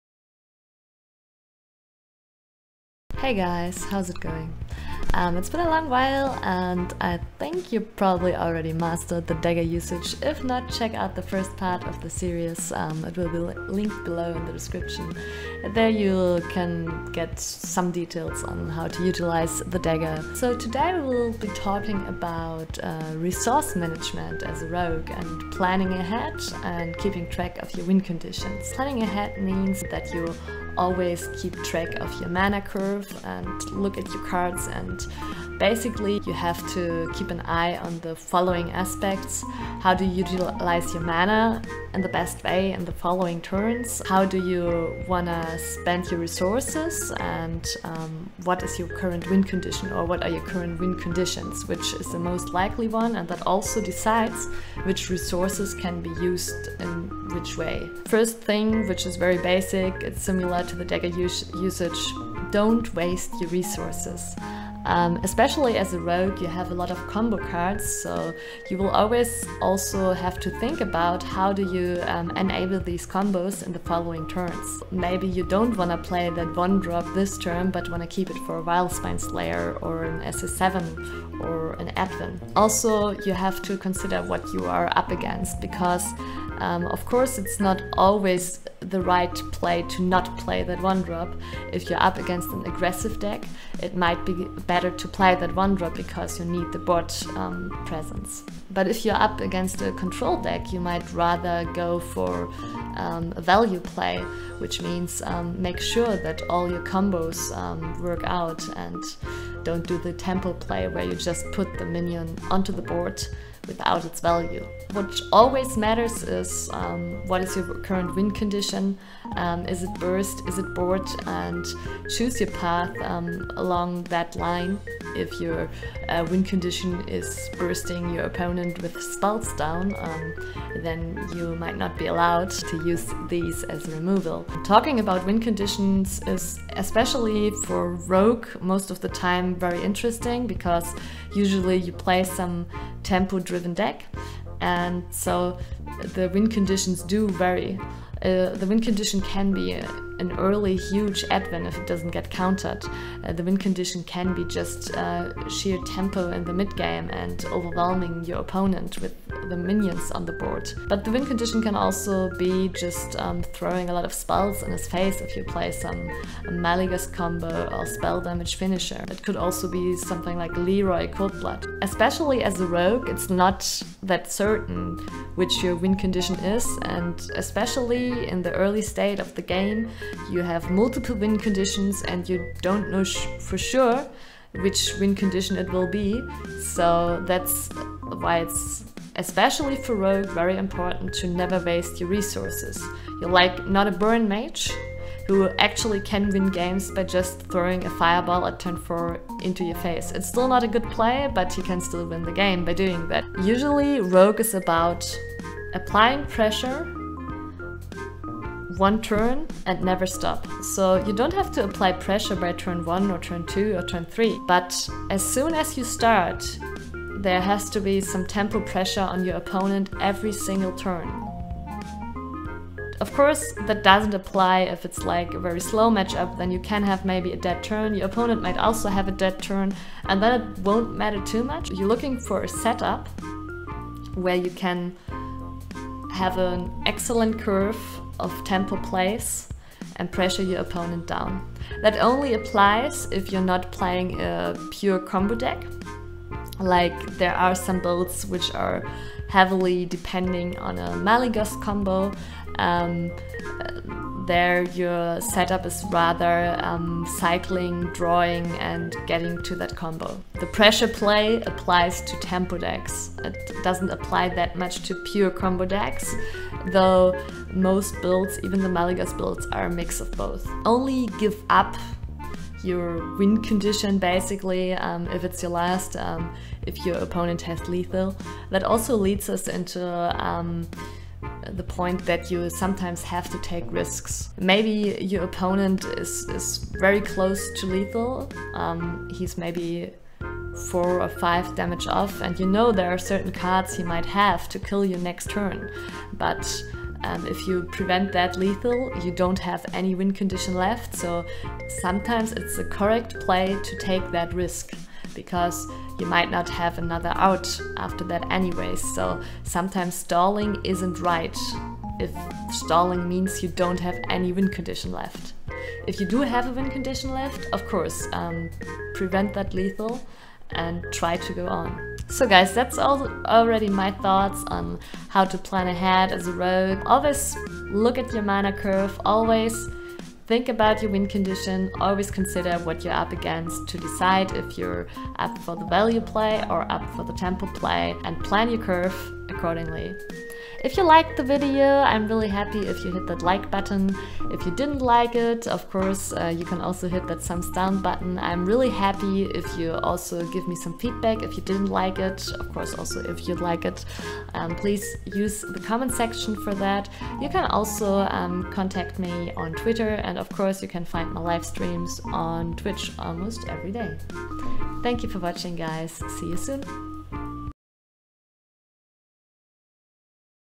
Rogue Basics #2 - Planning ahead
Part 2 in our "Rogue Basics" series. Looking at the first part, I think I have improved - not that stiff anymore XD Unfortunately the sun produced some very "bright" moments in this video, sorry about that.
I'm happy about any kind of feedback :)